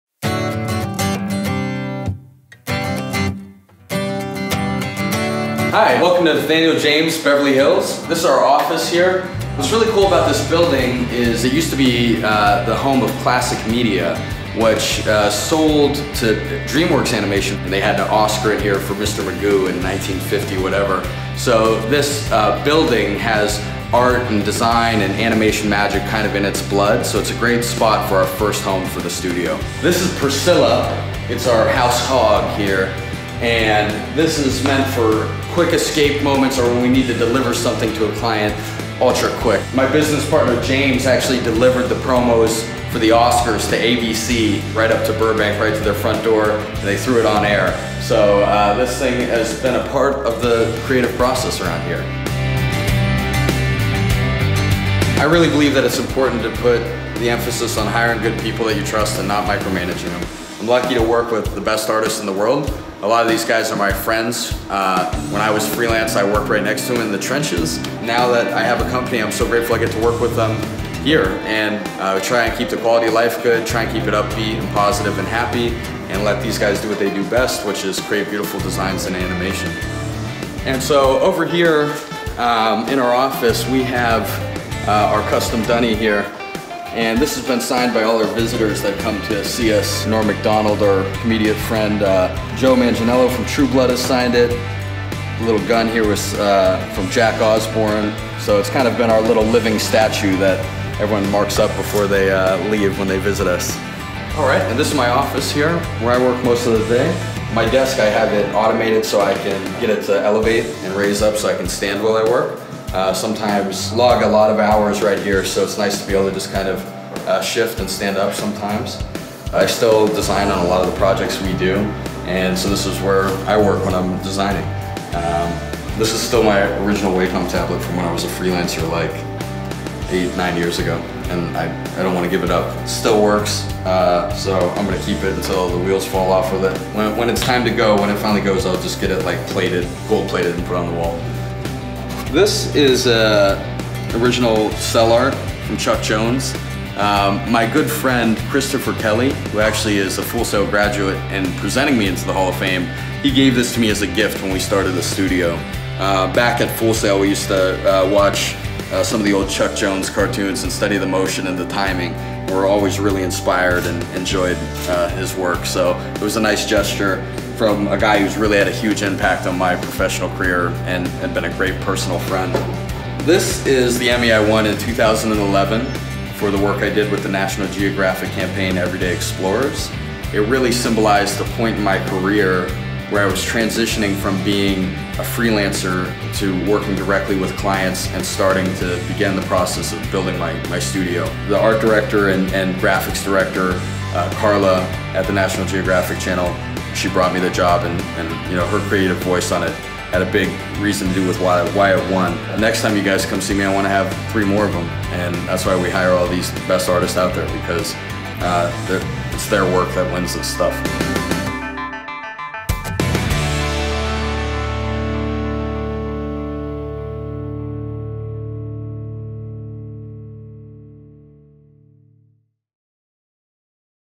A Tour of Nathaniel James with Hall of Fame Inductee Nate Howe
Note: As of June 2015, Nathaniel James has changed their name to Nathaniel How Studios.

Sixth Annual Hall of Fame inductee Nate Howe took us on a tour of his entertainment and branding agency, Nathaniel James. Nate, a 2002 Computer Animation grad, serves as the agency's Creative Director.

or check out Full Sail University's Hall of Fame